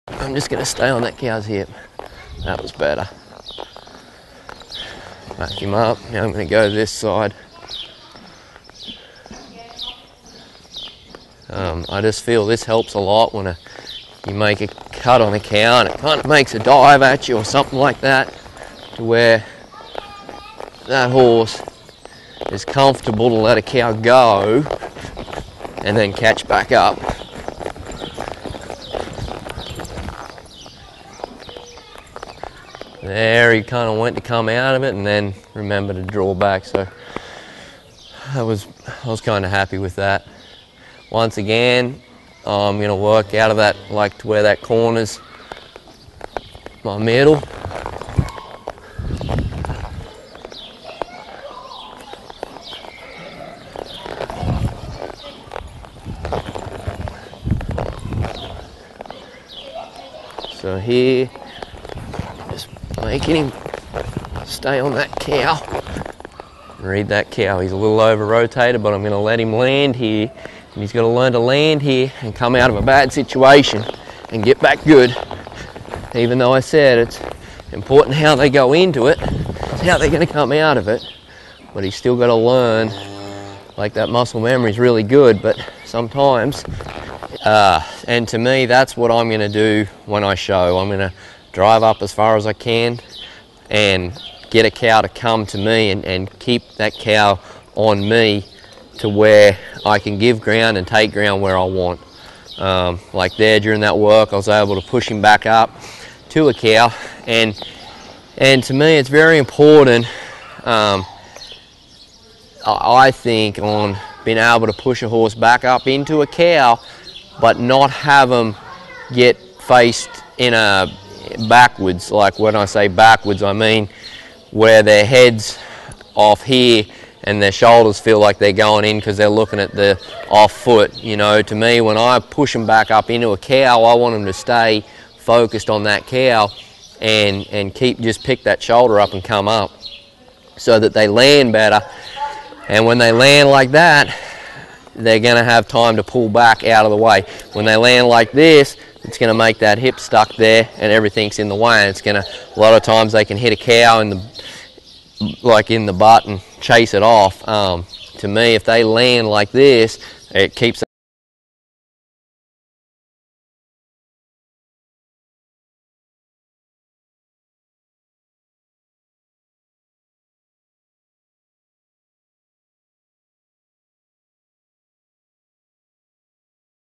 The Biggest Mistake People Make Working Their Cutting Horses Around With Geoffrey “Spud” Sheehan!
Spud works a 3yr old that is scared of cattle and he shows you how too:
*Hold a horse up against a cow.
*What foot to use.
*Why the horse has to be comfortable letting a cow go and then catching it.
*The biggest mistake people make when working around.
*Why it’s so important driving up in your cuts.
*How to let your horse have a win when working around.